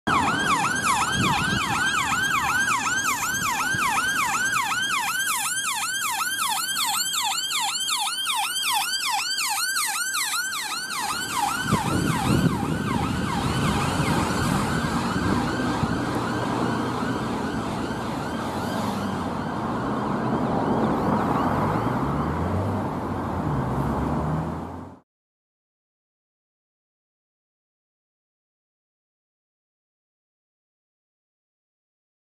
SCAS Driver Training Unit Responding in Basingstoke
A SCAS Drive Training Unit Responding past Basingstoke Fire Station.